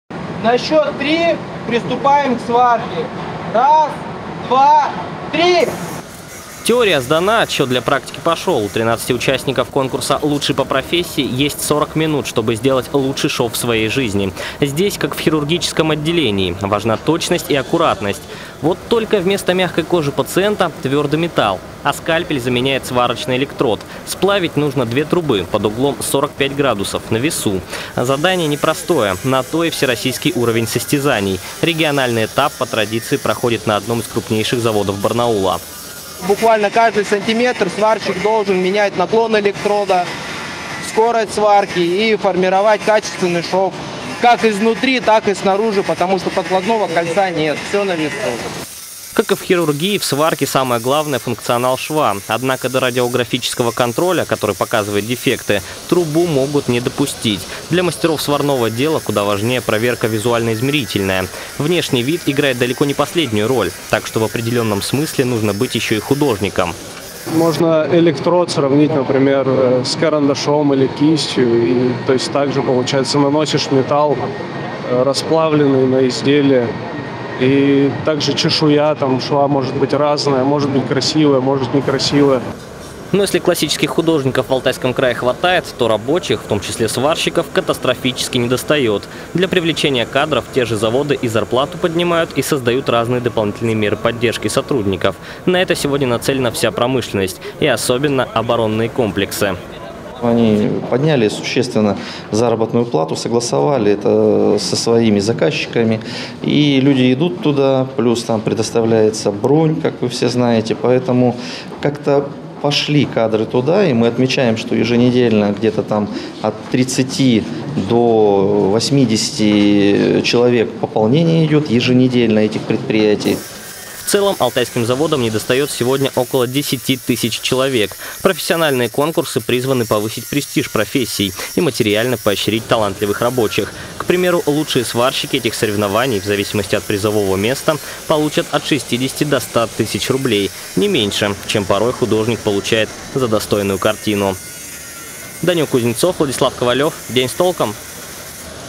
Лучшего сварщика края выбрали на профессиональном конкурсе
Самого первоклассного сварщика выбрали в Барнауле в рамках регионального этапа Всероссийского конкурса «Лучший по профессии». Эта работа требует и умений, и эстетики.